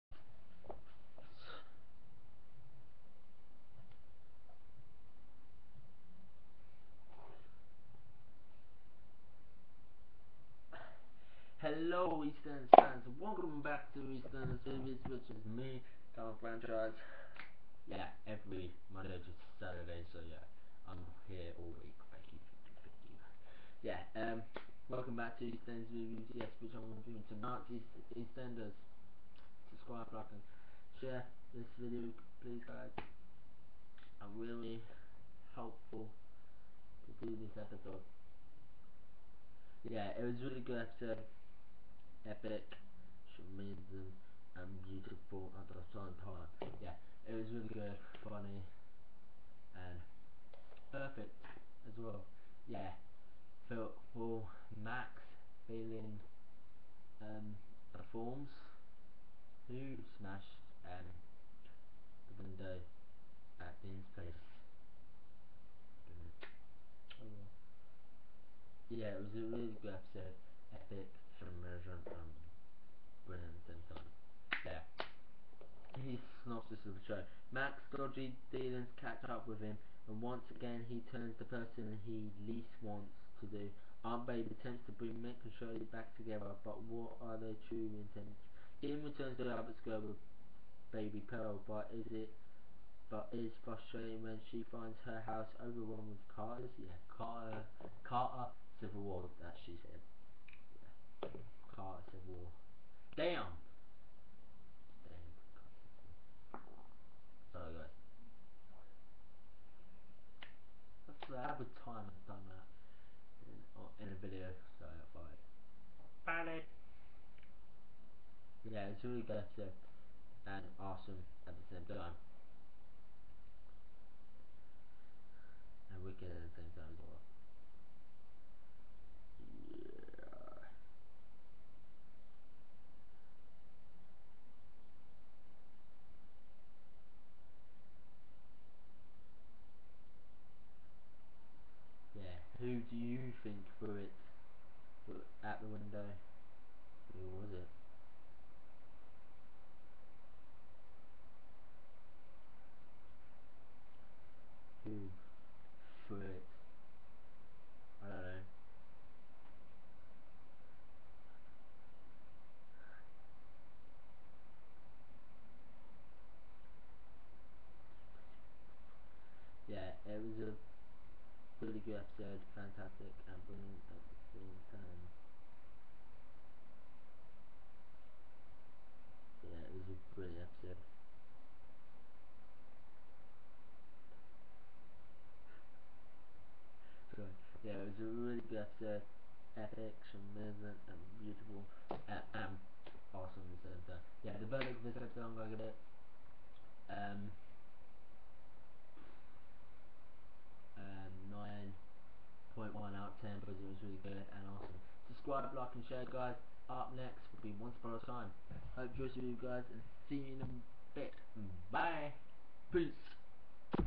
EastEnders Reviews: 27th April 2015
Tonight's episode was good,funny and epic EastEnders continues tomorrow on BBC One at 7:30pm i hoped you enjoyed this review guys see you in a bit bye.

Tonight's Cast
Phil Mitchell Steve McFadden
Max Branning Jake Wood
Abi Branning Lorna Fitzgerald
Ben Mitchell Harry Reid
Jay Brown Jamie Borthwick
Sharon Mitchell Letitia Dean
Denise Fox Diane Parish
Patrick Trueman   Rudolph Walker
Kim Fox-Hubbard Tameka Empson
Donna Yates Lisa Hammond
Kush Kazemi Davood Ghadami
Tina Carter    Luisa Bradshaw-White
Shirley Carter Linda Henry
Dean Wicks Matt Di Angelo
Andrew 'Buster' Briggs Karl Howman
Mick Carter Danny Dyer
Linda Carter Kellie Bright
Babe Smith Annette Badland
Alfie Moon Shane Richie
Billy Mitchell Perry Fenwick
Jane Beale Laurie Brett
Ian Beale Adam Woodyatt
Carol Jackson Lindsey Coulson
Whitney Dean Shona McGarty
Lola Pearce Danielle Harold
Kat Moon Jessie Wallace
Stacey Branning Lacey Turner
Charlie Cotton Declan Bennett
Ritchie Scott Sian Webber
Karin Smart Denise Van Outen
Gareth Hunt Ojan Genc
Writer Jonny O'Neill
Director Rebecca Gatward
Executive Producer Dominic Treadwell-Collins